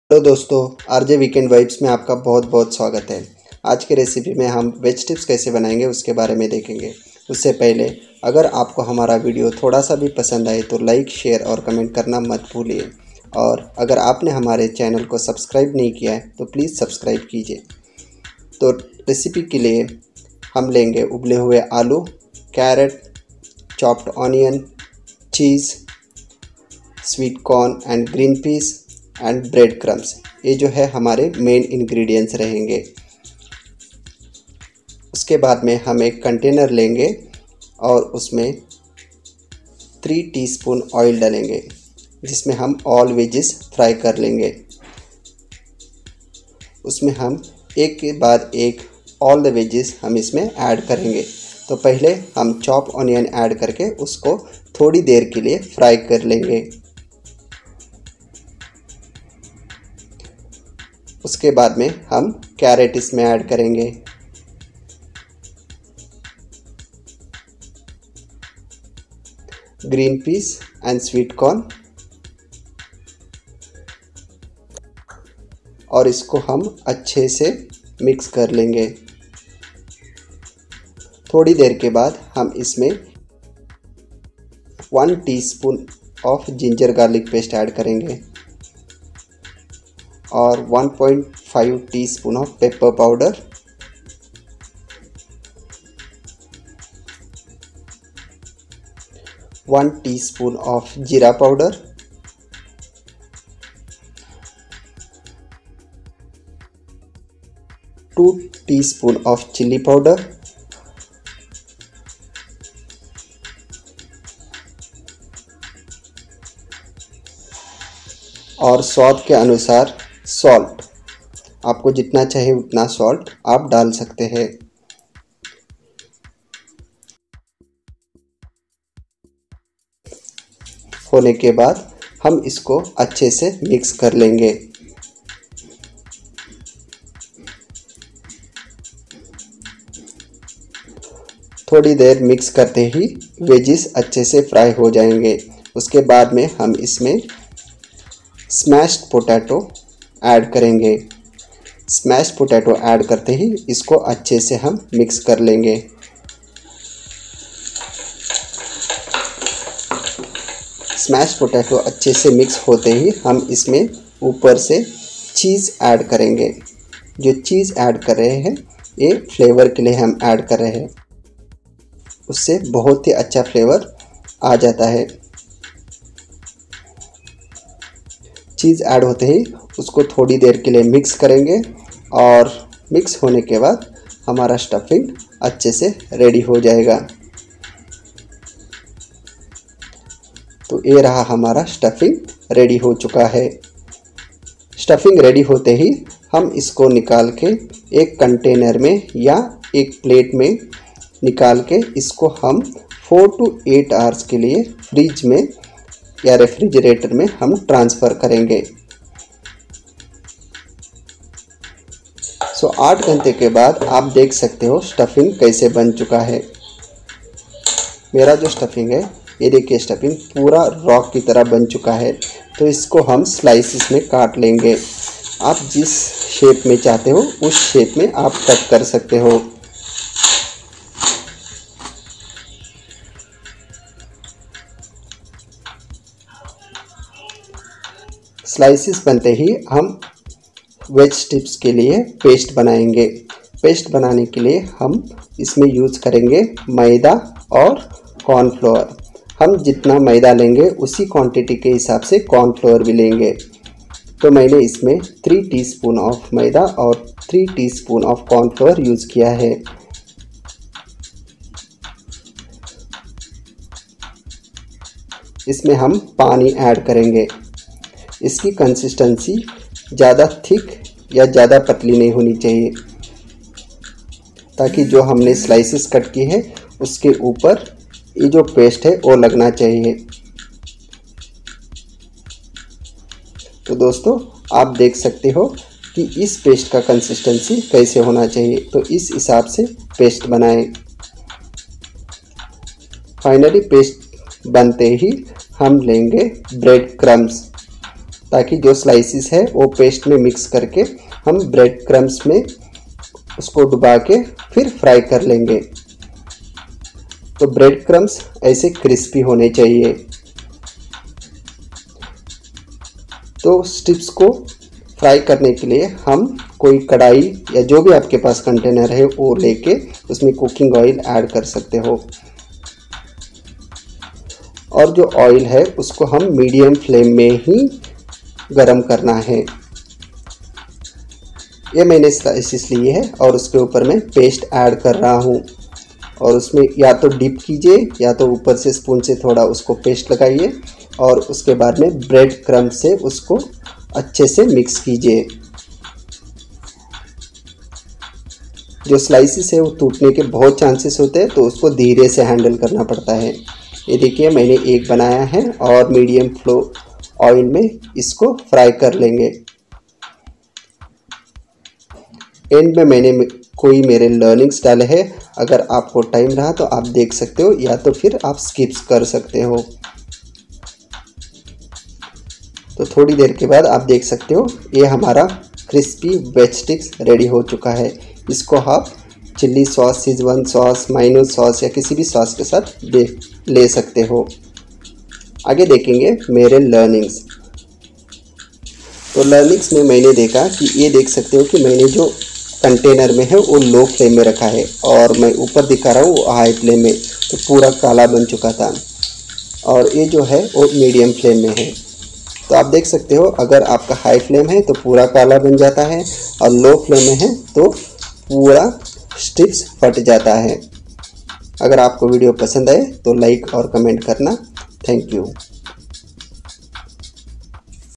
KFC style Veg Strips ! Crunchy vegetable nuggets!
Ingredients

1. Potato - 2/3
2. Cartot 3
3. Sweet corn 1cup
4. Green peas 1cup
5. Maida 3 tablespoon
6. Corn flour 3 tablespoon
7. Ginger garlic paste 1 teaspoon
8. Salt to taste
9. Oil for frying
10. Bread crumbs 1 cup
11. Jeera Powder 1tsp.
12. Pepper Powder 1.5 tsp.
13.Chilli Powder 2/3 tsp.